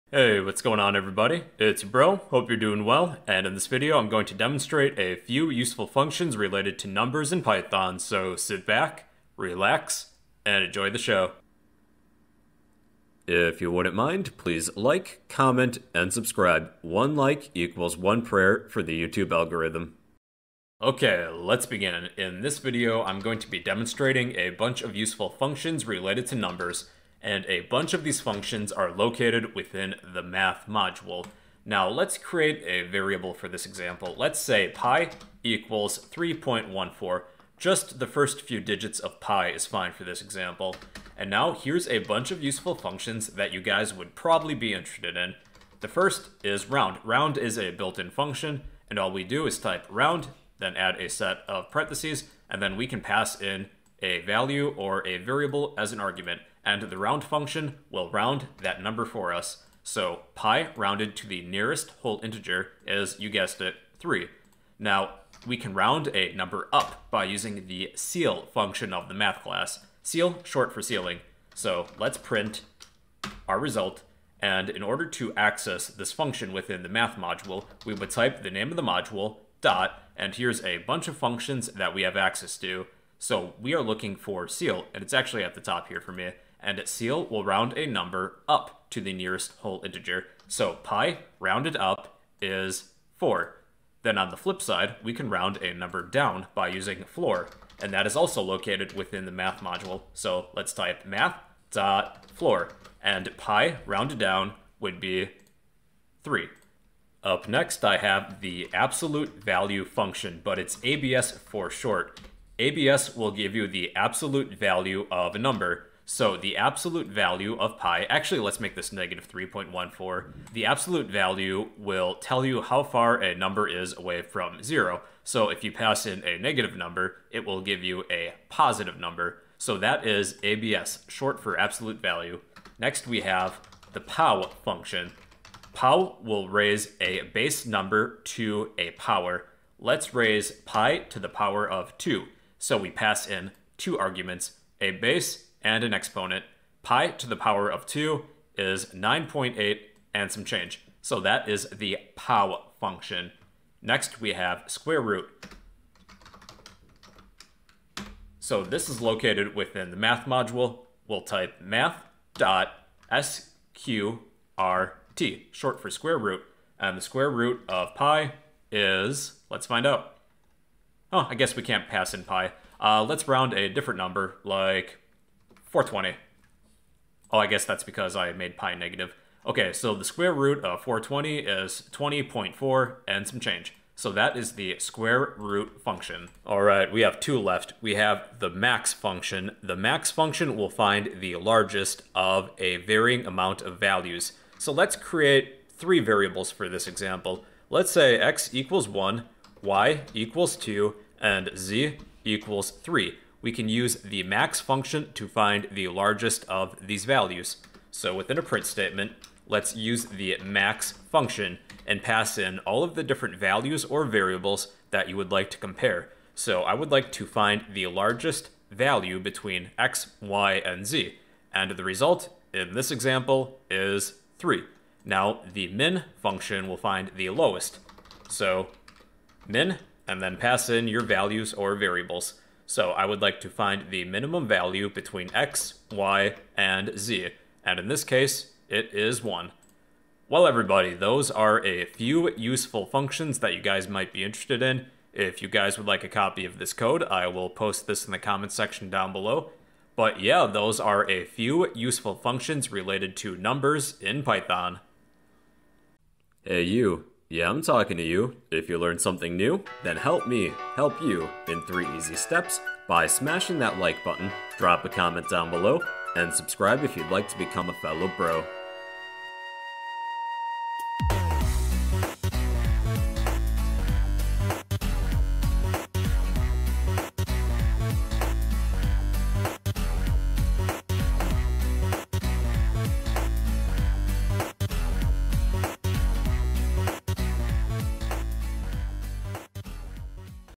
Python math functions 🧮
python math functions tutorial example explained

import math

pi = 3.14
x = 1
y = 2
z = 3

print(round(pi))
print(math.ceil(pi))
print(math.floor(pi))
print(abs(pi))
print(pow(pi,2))
print(math.sqrt(420))
print(max(x,y,z))
print(min(x,y,z))